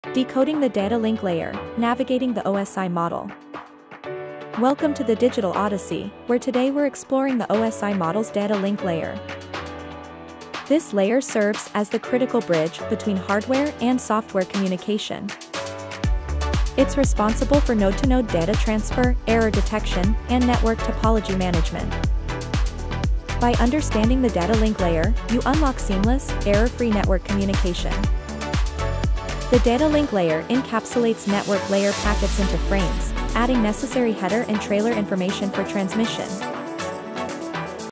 Layer Two - Data Link Layer
Uncover the intricacies of the OSI Model's Data Link Layer, where data packets are framed and addressed for reliable communication. Dive into the world of MAC addresses, error detection, and flow control, as we explore the backbone of data transfer in computer networks.